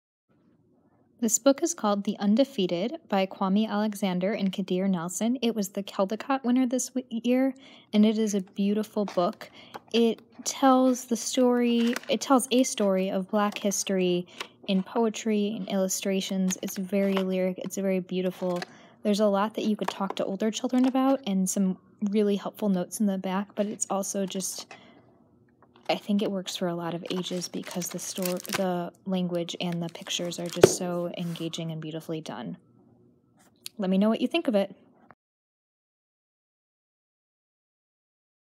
New Picture Book: The Undefeated by Kwame Alexander and Kadir Nelson
At Decorah Public Library: Kwame Alexander and Kadir Nelson